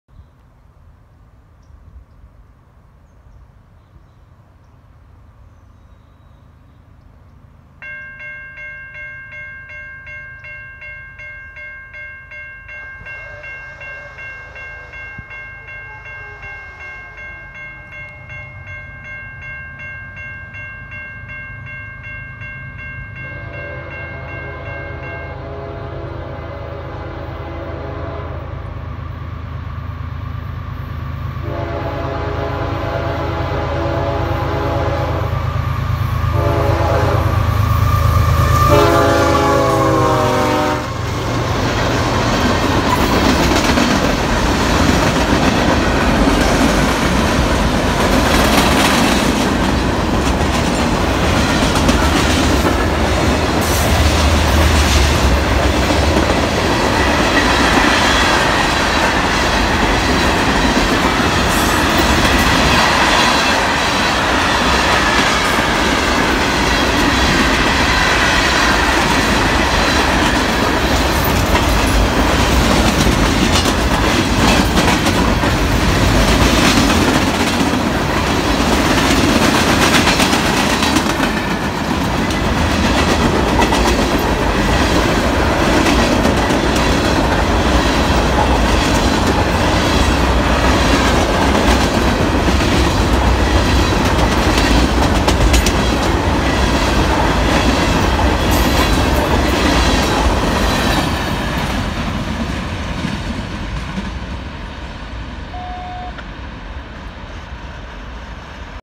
UP 5281 Leads NS 373 in Dover, GA
Foreign Power lead this train from the Dillard Yard in Savannah to Brosnan Yard in Macon.
Power:
UP 5281–GE C45ACCTE—2nd gen K5HL horn
NS 7596–GE ES44DC
Date: 3/11/19